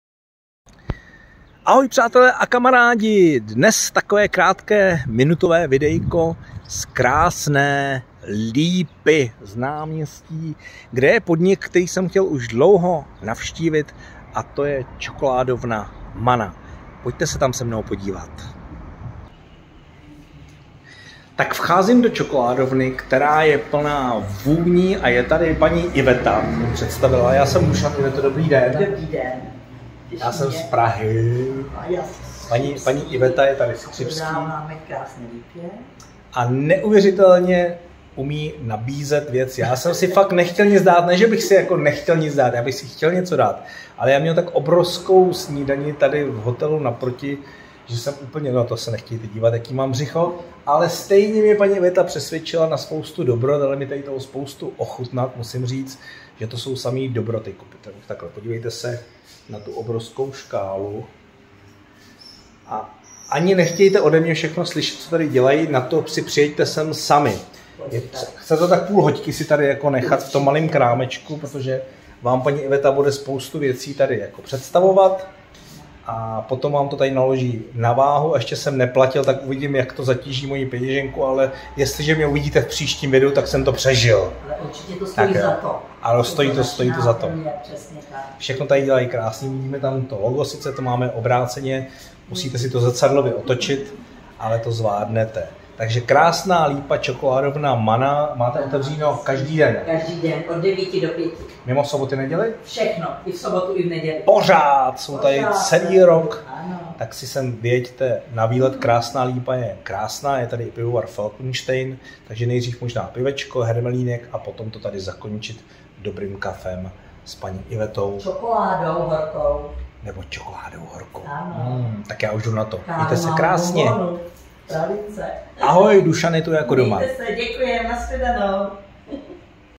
Čokoládovna Mana v Krásné Lípě
Dnešní video bude další z tipů na výlet a bude vonět čokoládou. Podíváme se totiž do výrobny čokoládovny na severu Čech.